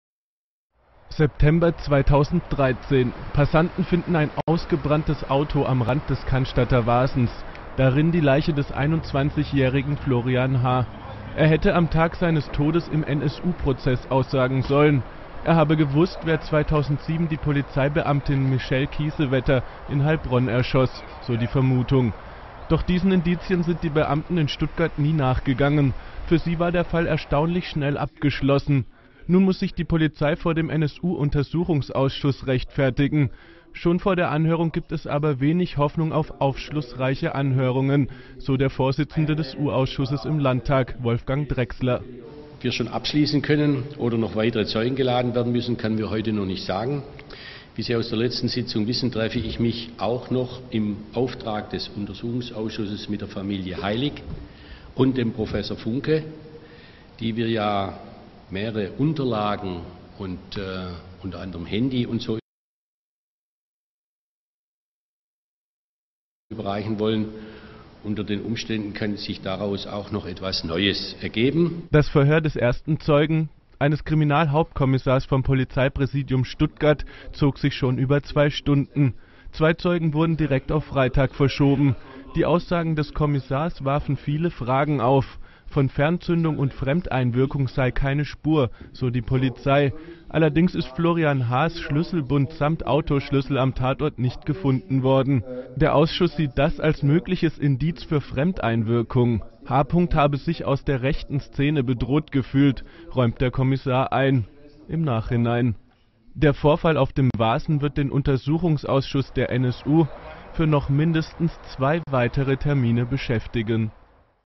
florian heilig 16.9.2013
Tatortaufnahmen mit Leiche im Auto. Wie am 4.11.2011 in Eisenach.